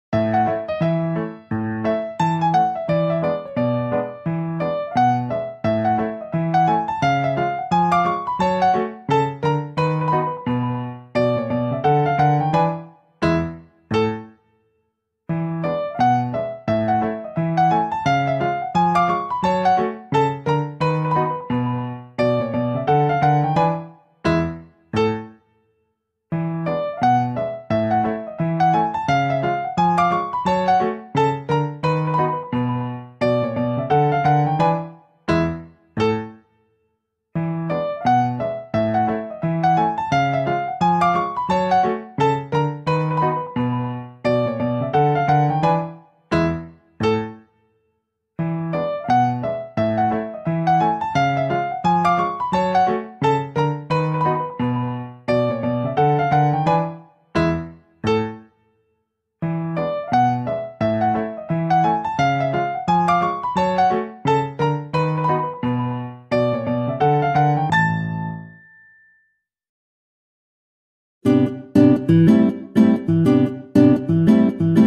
What Beanie Boos think of Cats sticking out their tongues at them 😜😆
Cats stick out their tongues at beanie boos and not all are impressed 😊😁